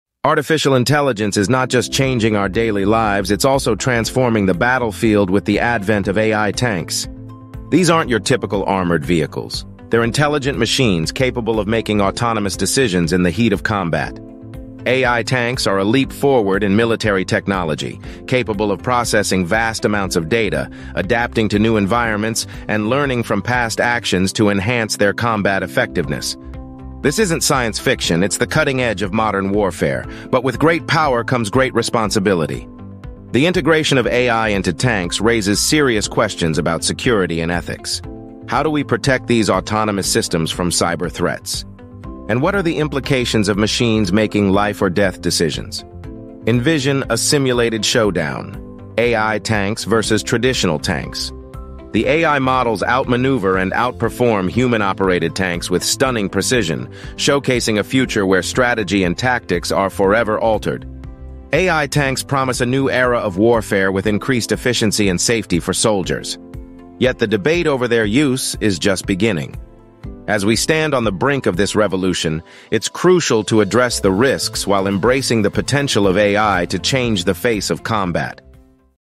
The Impact of AGI on Warfare: AI Tanks
Discover the future of warfare with autonomous technology as we delve into the world of AI tanks in battle. In this video, we explore the impact of AI on modern warfare and focus on the development of autonomous AI tanks. Learn about how these advanced machines are changing the landscape of military operations and the implications they have for the future of conflict.